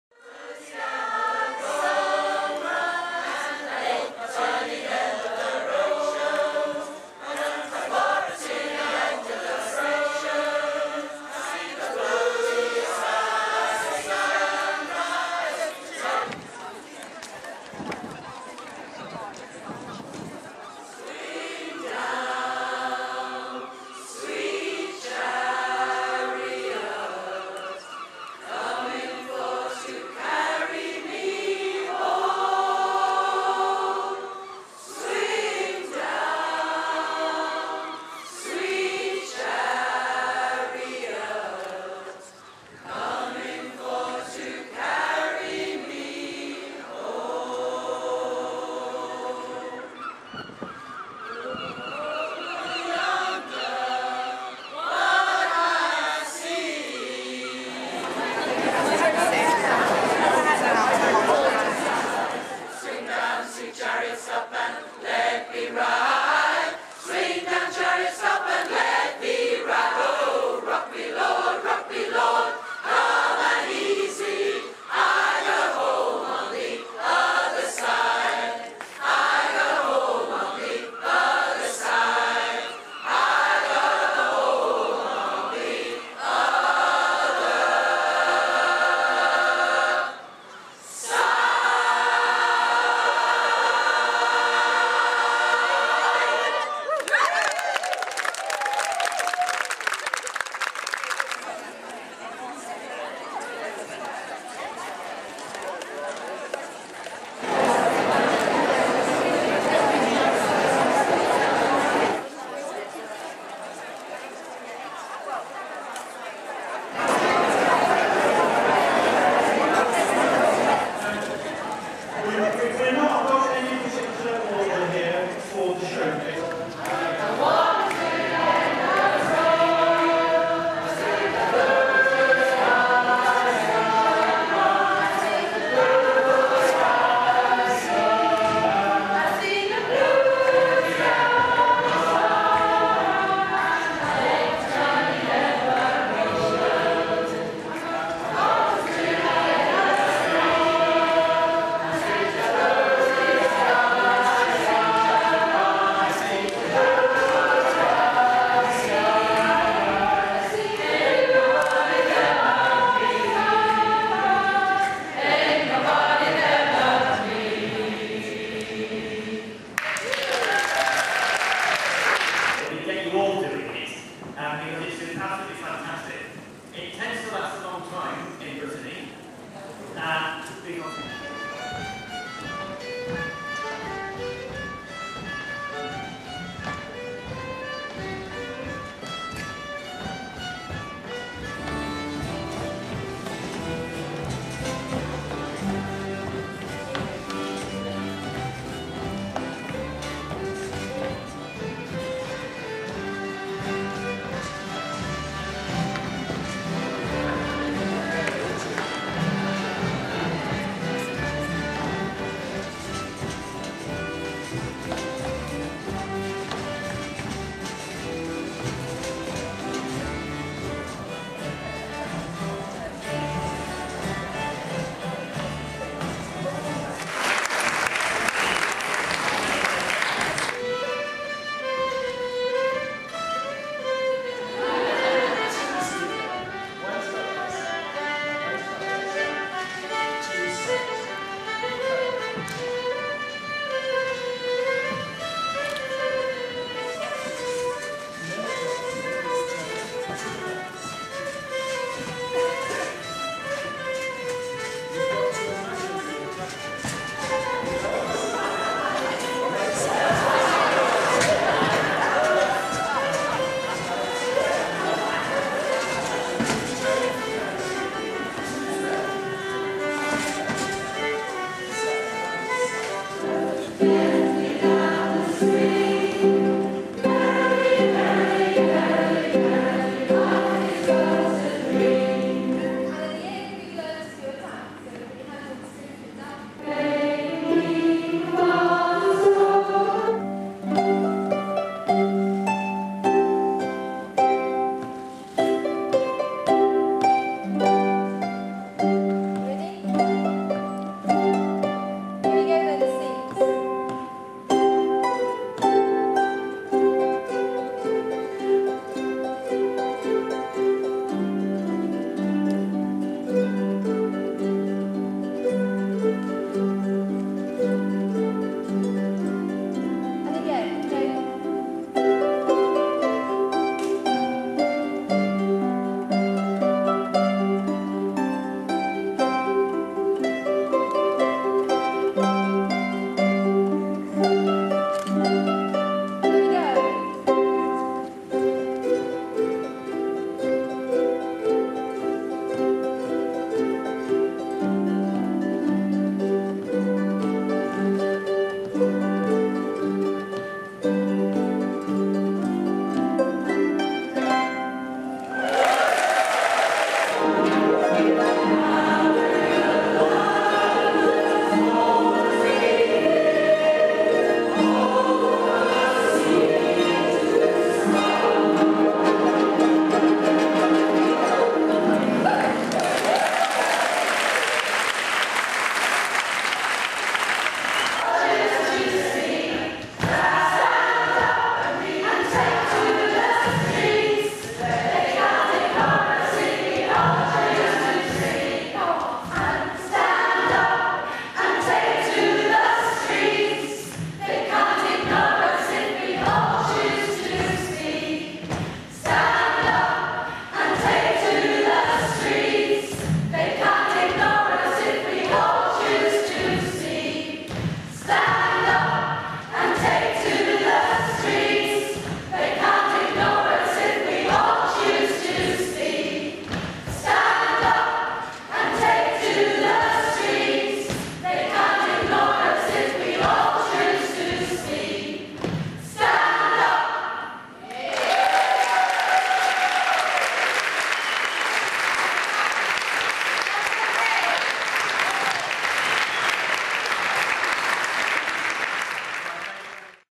Streetchoirs 2013 - Sunday Morning Workshops & Showcase
Sunday morning Workshops and Showcase at the 2013 National Streetchoirs Festival in Aberystwyth.